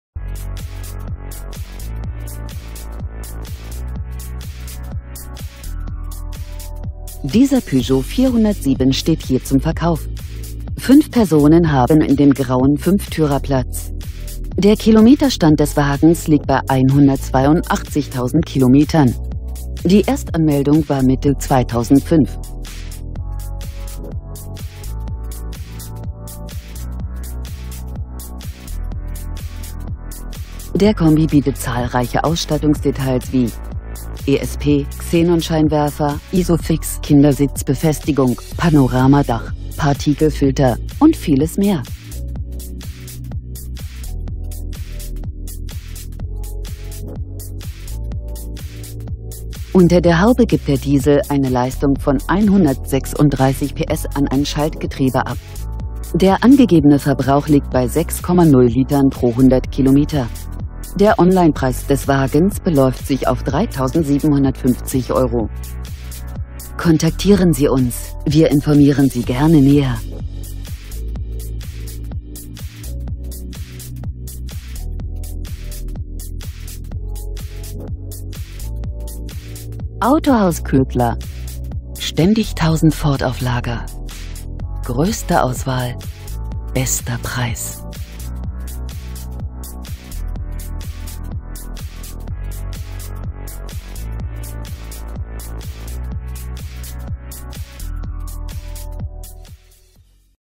Peugeot 407 407 SW HDi 135 Platinum
Endpreis: € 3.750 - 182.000 km - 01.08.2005. Weitere Details:
Kombi, Power{kw=100.0} KW, Schaltgetriebe, grau